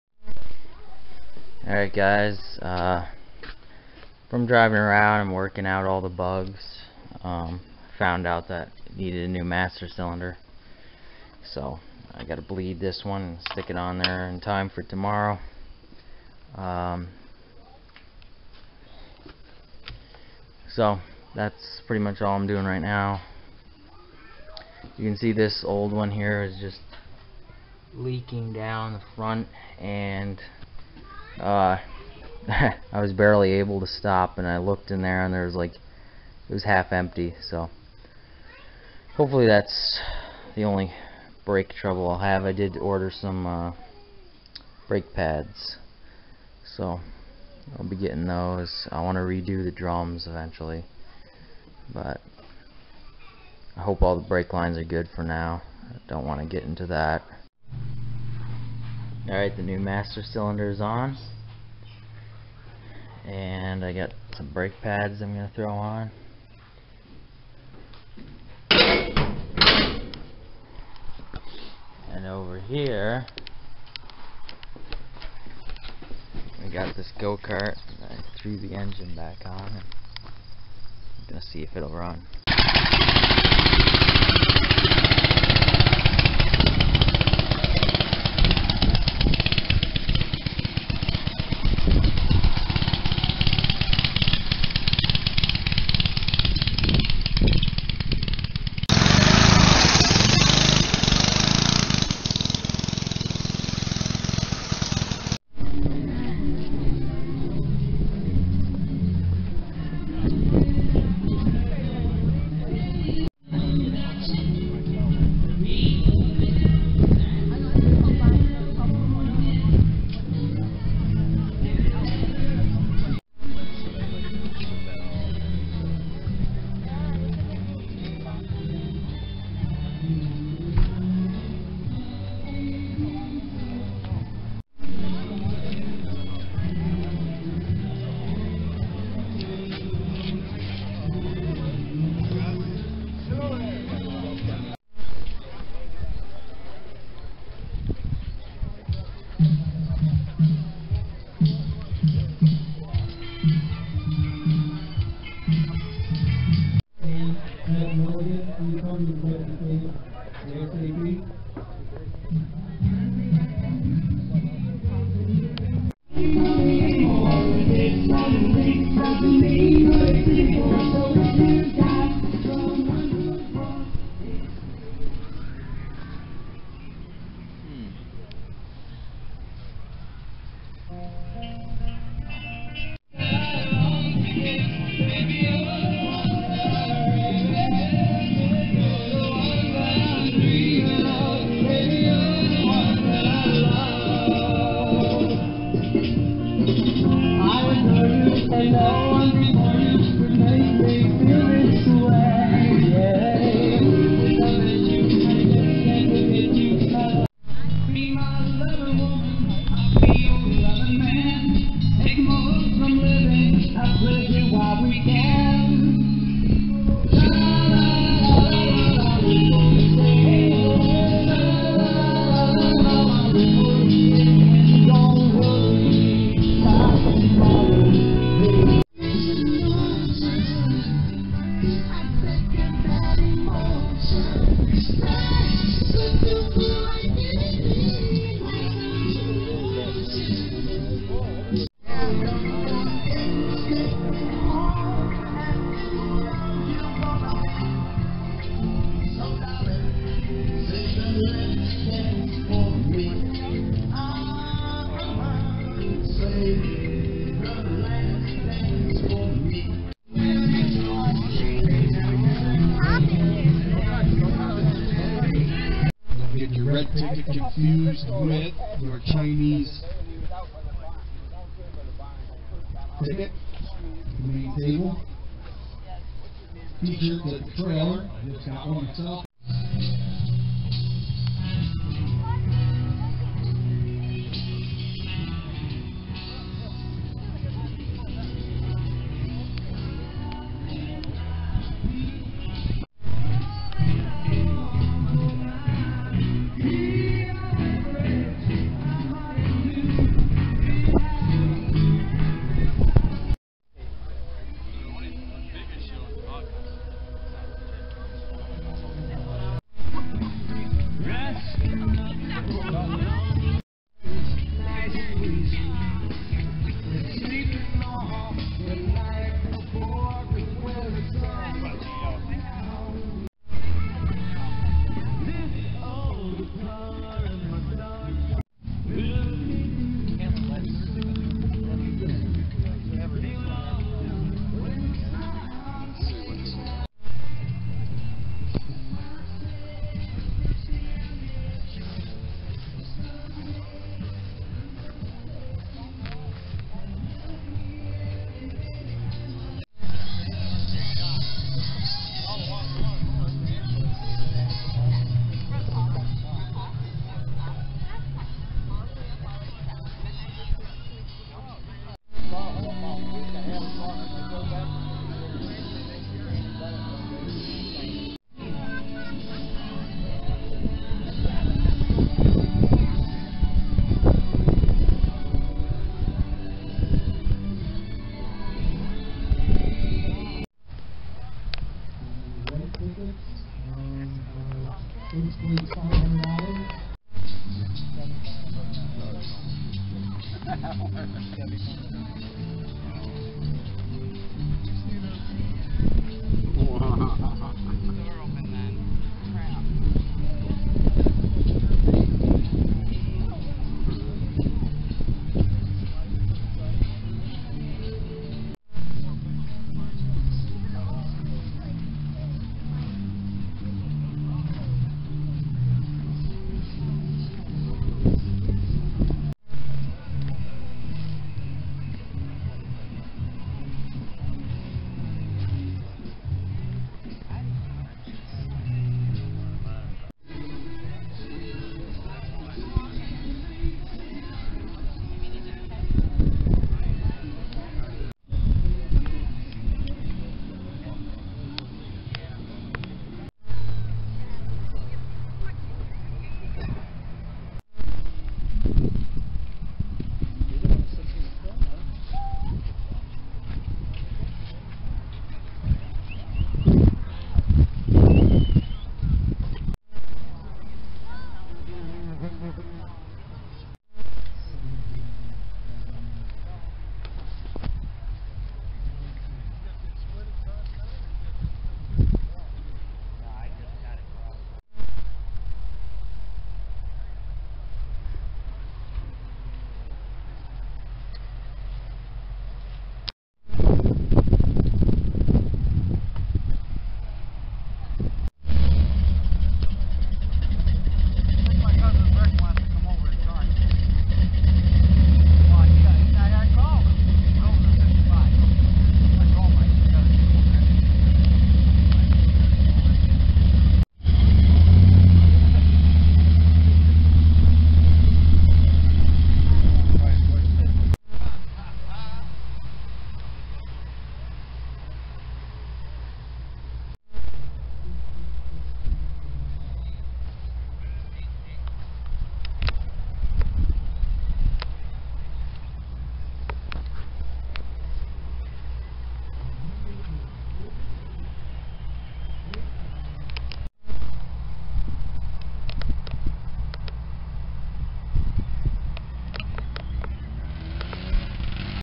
Go kart runs - New master cylinder - car show
Go kart runs - New master cylinder - and car show i missed because of a split brake hose